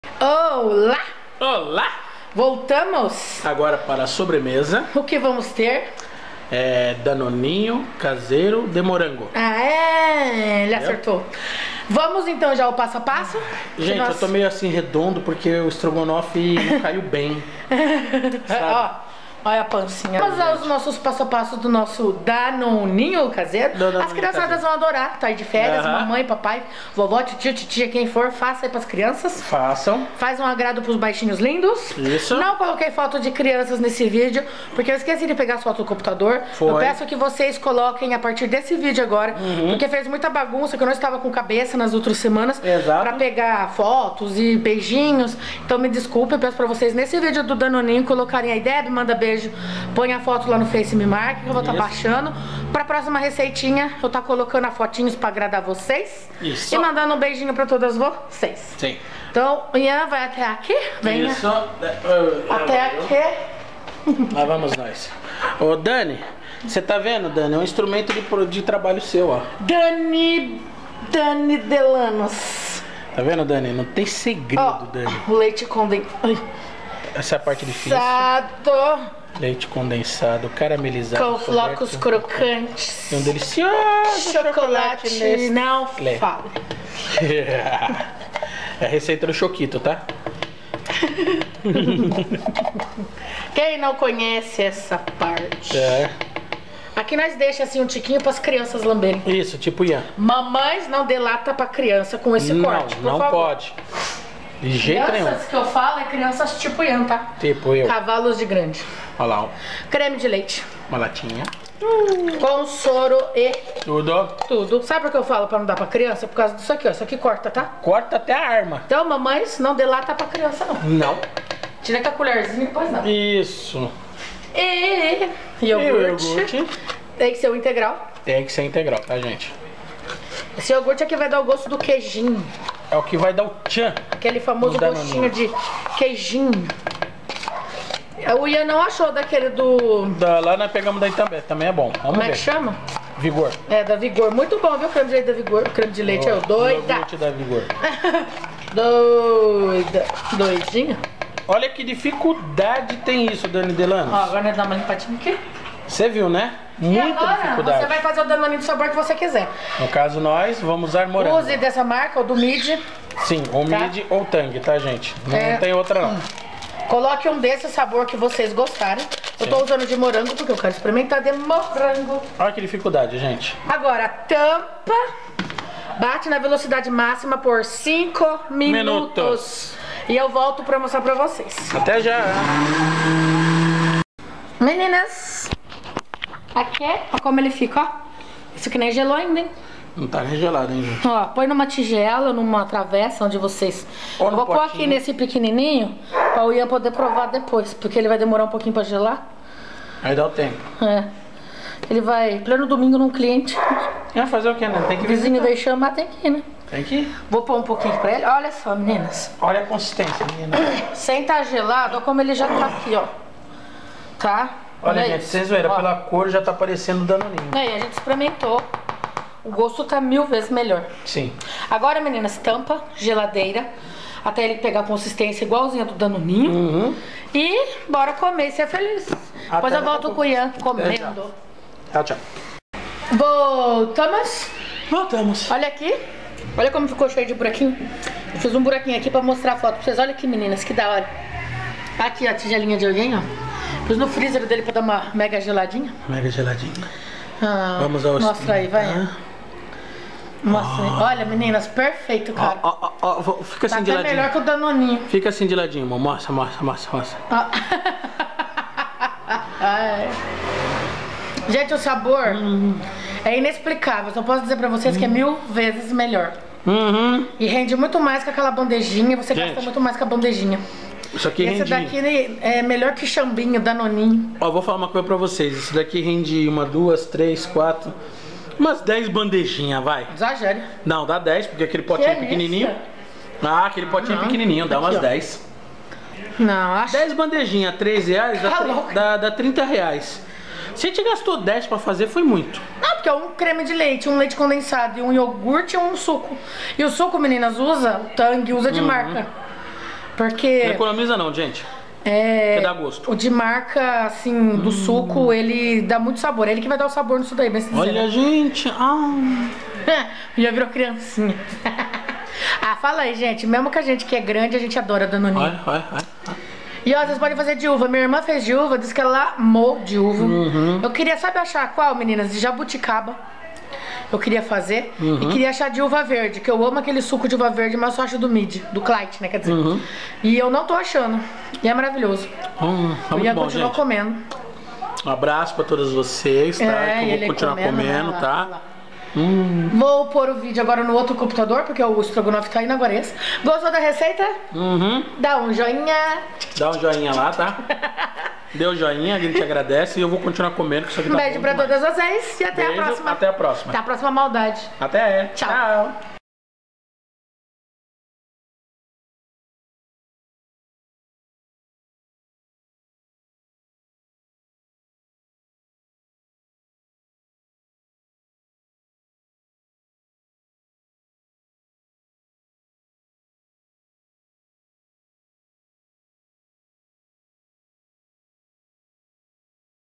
Danoninho Caseiro
INGREDIENTES :)

1 LATA DE LEITE CONDENSADO
1 LATA DE CREME DE LEITE
1 COPO IOGURTE INTEGRAL NATURAL
1 SUCO EM PÓ TANG

MODO DE FAZER ASSISTA AO VIDEO  :)